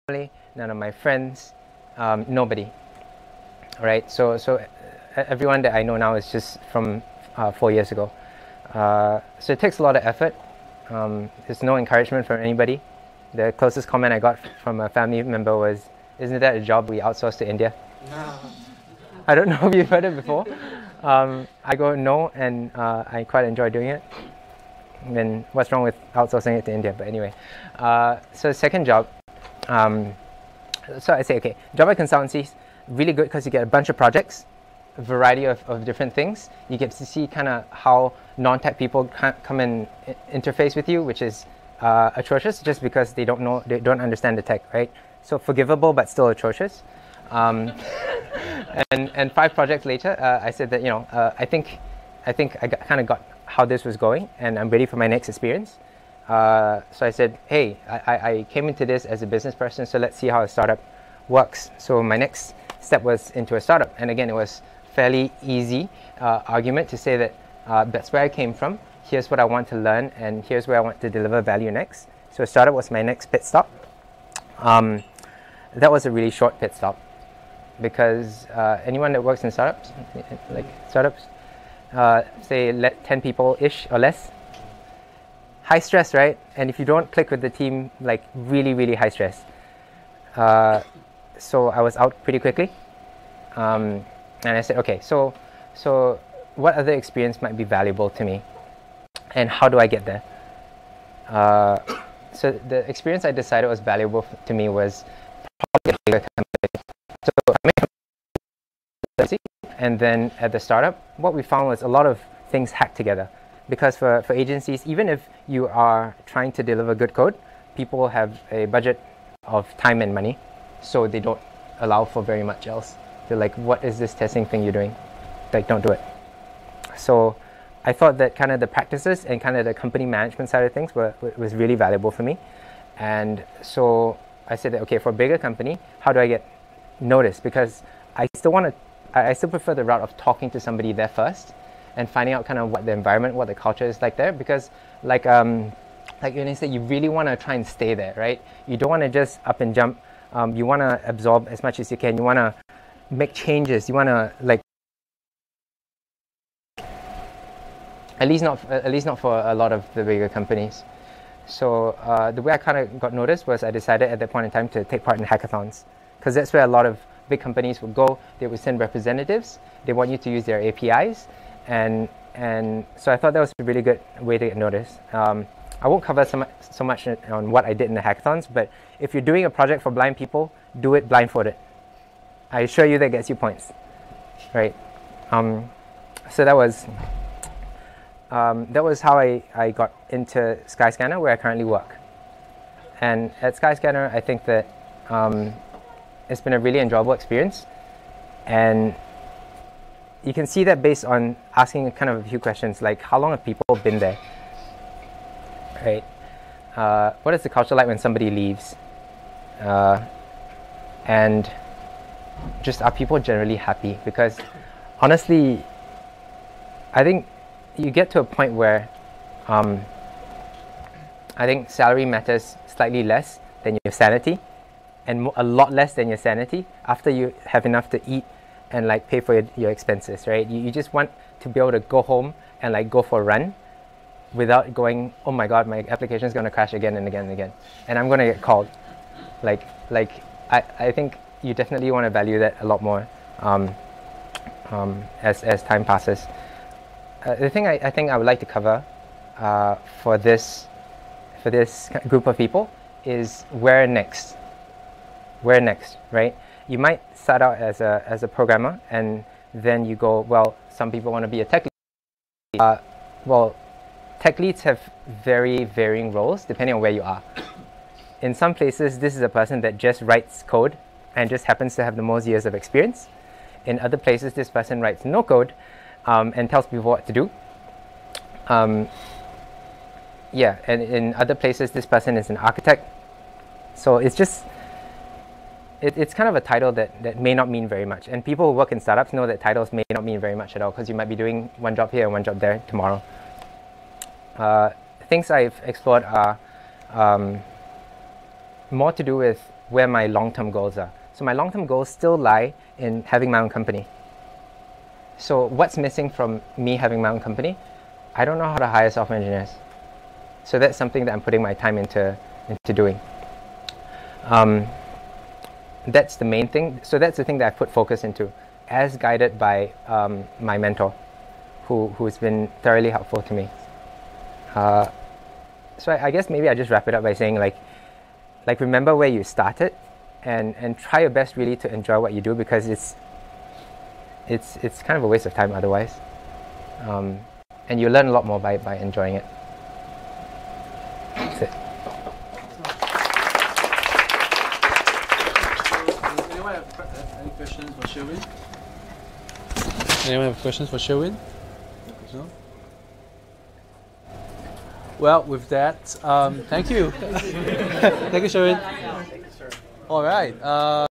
Personal Journey of Sherwyn Goh - Junior Developers Singapore
Speaker:  Sherwyn Goh, Software Engineer, Skyscanner

(Due to a technical issue, the first few minutes of this talk is not recorded)

Sherwyn in his own words:"Ten years ago, I used to teach people to lift weights. Then I made Mexican food. Now I write programs on a computer for a living."

Recorded by: Yeo Kheng Meng